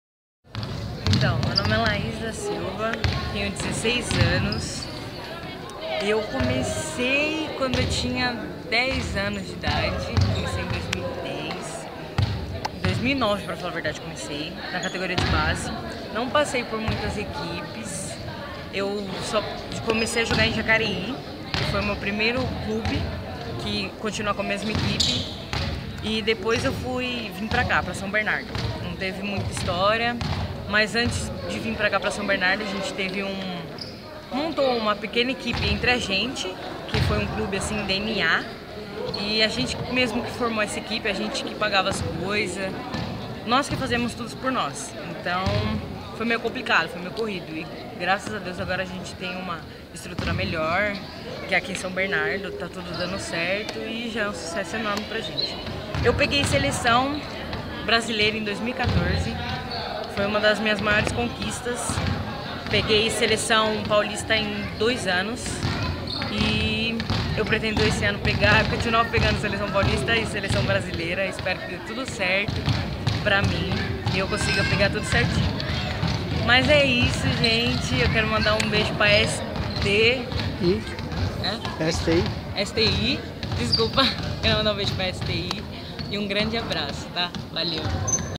Jogadora Lais do ADC São Bernardo fala ao STI Esporte
Imagem foi gerada antes da vitória do ADC São Bernardo sobre o Internacional de Santos por 72 a 58 no dia 20/05